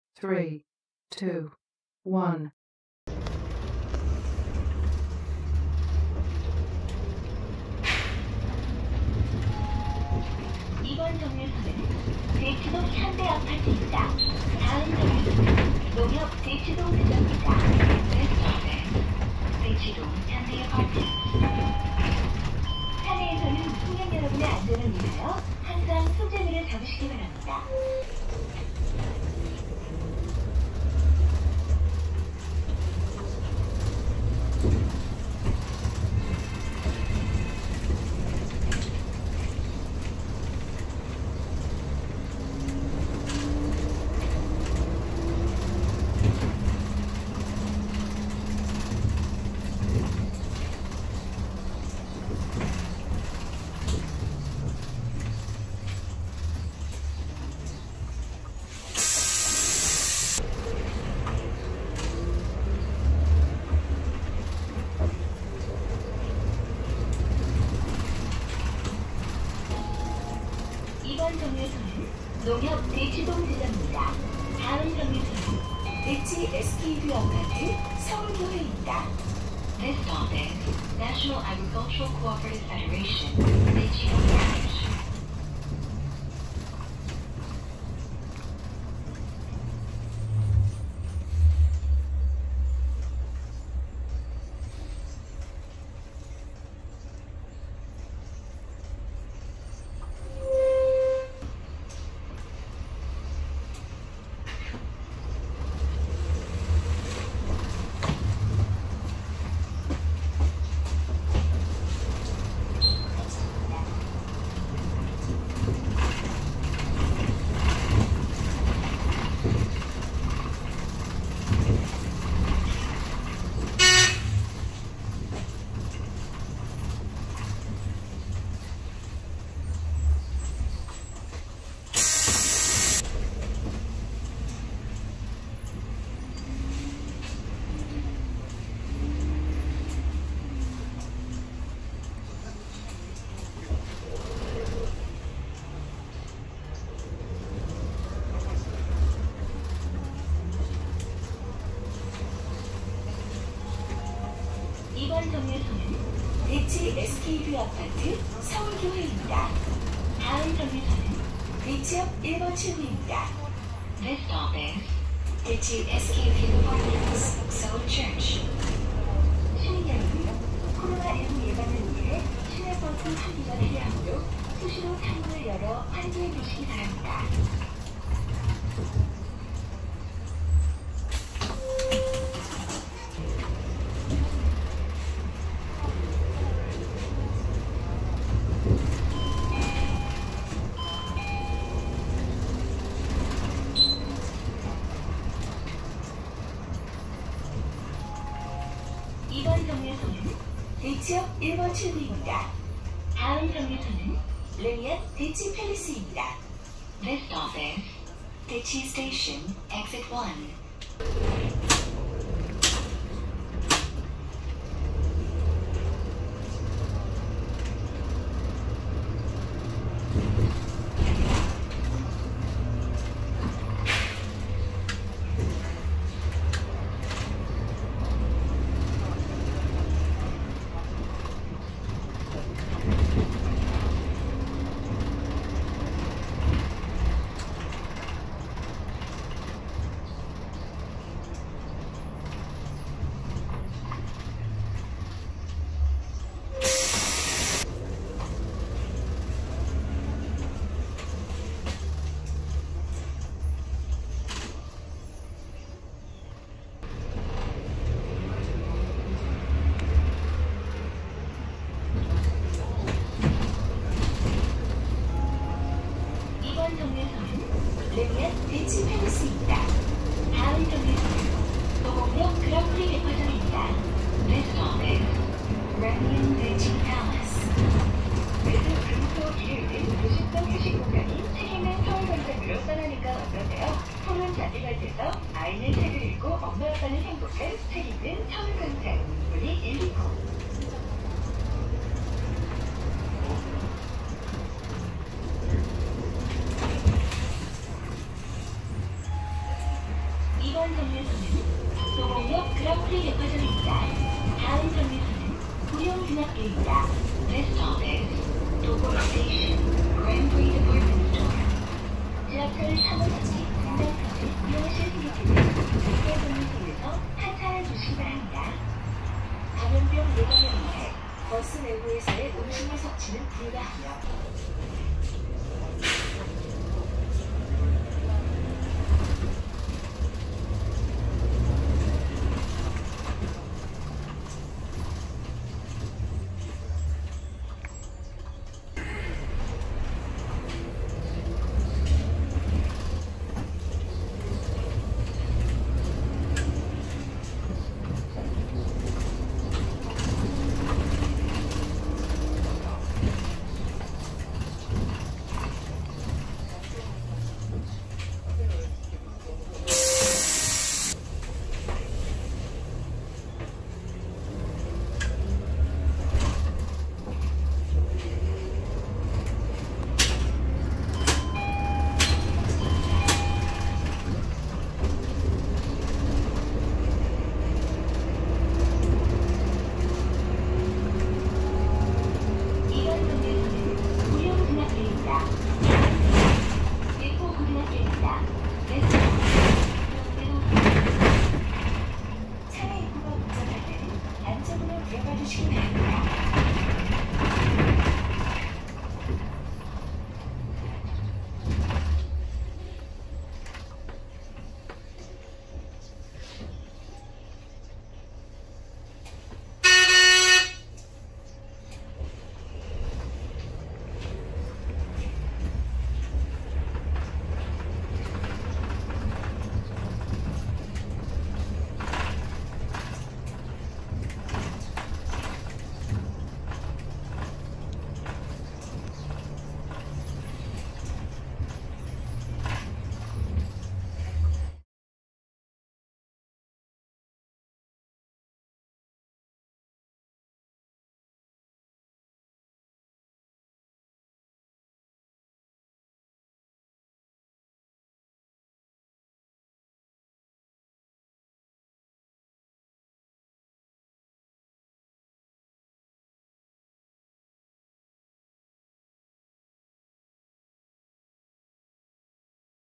서울 2413번 버스 주행영상 | Seoul Bus 2413 Driving Tour | 휘문중고등학교~구룡중학교앞 | 갤럭시Z폴드2 | 4K 60fps
안녕하세요. 오늘은 갤럭시Z폴드2로 촬영한 서울의 시내버스 투어를 진행해보겠습니다.
서울 2413번 버스는 서울 강남/강북을 운행하는 버스로,
성수동차고지에서 출발해서 뚝섬역, 영동대교, 삼성역, 도곡역을 거쳐 개포파크빌리지까지 운행합니다.

버스 앞유리를 통해 보이는 서울 도심의 풍경을 즐겨보시기 바랍니다.
버스 운전기사님의 주행 능력을 살펴보는 것도 소소한 재미가 될 수 있겠네요.

Time Stamp.
00:00 서울 2413번 버스 주행 시작
00:52 대치동현대아파트 (23-262)
02:08 농협대치지점 (23-224)
02:57 대치SK.VIEW아파트.서울교회 (23-225)
04:02 대치역1번출구 (23-226)
04:42 래미안.대치.팰리스 (23-227)
05:55 그랑프리백화점 (23-456)
07:04 구룡중학교앞 (23-334)
07:14 Credits

* 카메라 : Insta360 One X 2, iPhone 12 Mini (2020), Galaxy Z Fold 2 (2020)
* 마이크 : Zoom H1, Blue Yeti Nano
* 편집 프로그램 : Final Cut Pro X
* 짐벌 : Osmo Mobile 4

Seoul Bus 2413 Driving Tour | Whimoon Middle/High School~Guryong Middle School | Galaxy Z Fold 2 | 4K 60fps

Hello, Today, I'll have a city bus tour in Seoul that was filmed with Galaxy Z Fold 2.
Seoul Bus No. 2413 runs across Gangnam/Gangbuk, Seoul.
It departs from Seongsu-dong Garage and runs through Ttukseom Station, Yeongdong Bridge, Samseong Station, and Dogok Station to Gaepo Park Village.

Enjoy the view of downtown Seoul through the bus windshield.
It would be fun to look at the driving ability of the bus driver.

Time Stamp.
00:00 Seoul Bus No.2413 starts running
00:52 Daechi-dong Hyundai Apt (23-262)
02:08 Nonghyup Daechi Branch (23-224)
02:57 Daechi SK.VIEW Apt. Seoul Church (23-225)
04:02 Daechi Station Exit No.1 (23-226)
04:42 Raemian.Daechi.Palace (23-227)
05:55 Grand Prix Department Store (23-456)
07:04 In front of Guryong Middle School (23-334)
07:14 Credits

* Camera : Insta360 One X 2, iPhone 12 Mini (2020), Galaxy Z Fold 2 (2020)
* MIC : Zoom H1, Blue Yeti Nano
* Edit Program : Final Cut Pro X
* Gimbal : Osmo Mobile 4